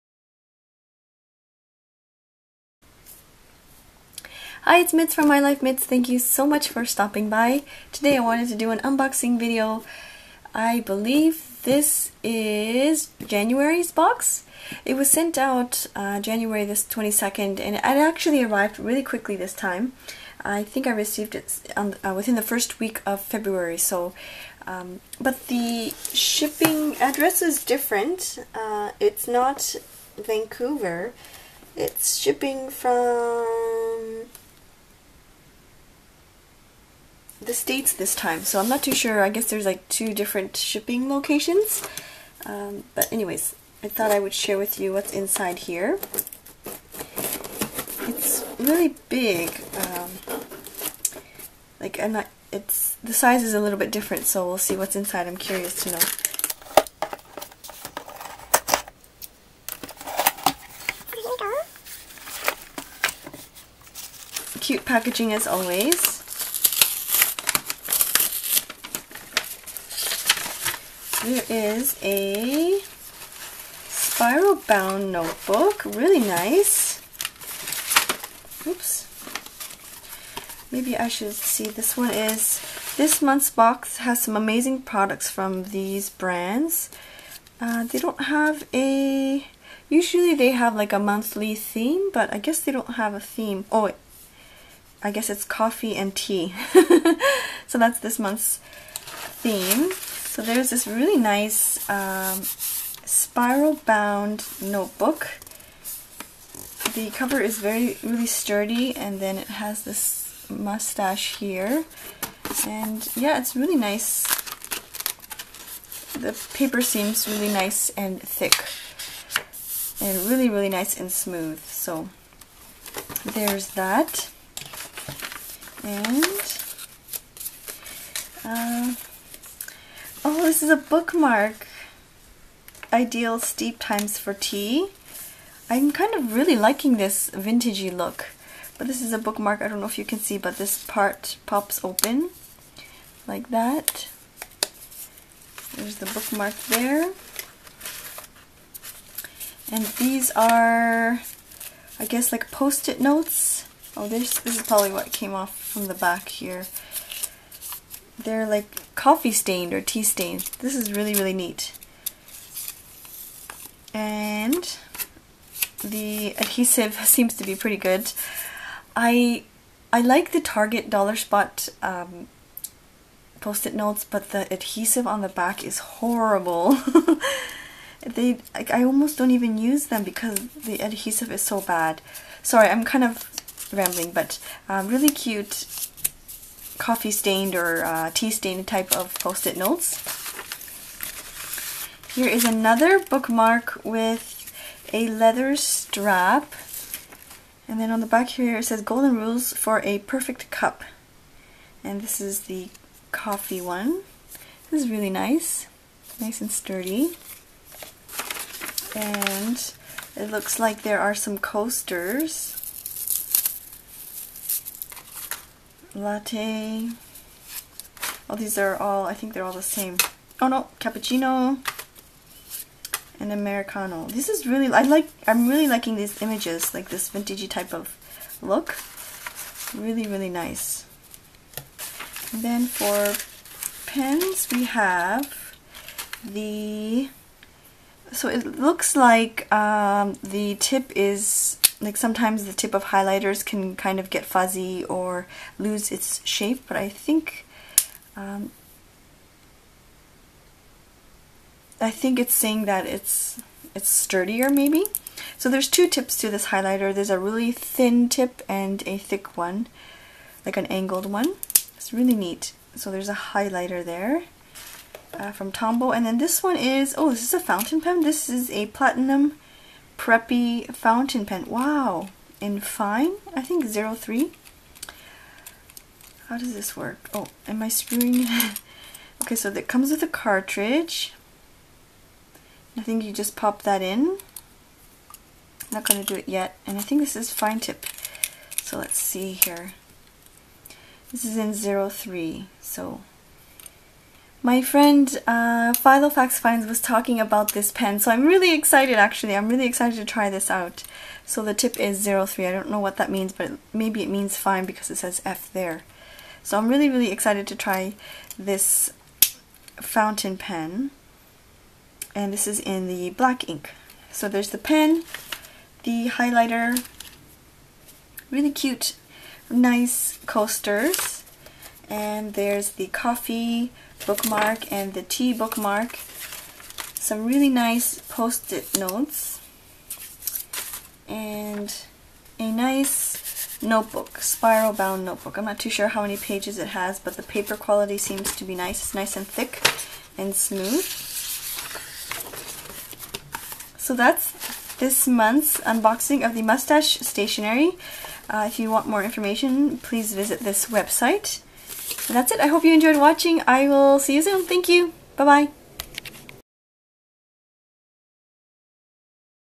Moustache Stationery Unboxing January 2016
This month's Moustache Stationery Theme is: Coffee and Tea

Note: This subscription box was kindly provided to me by Moustache Stationery. All opinions are honest and are my own.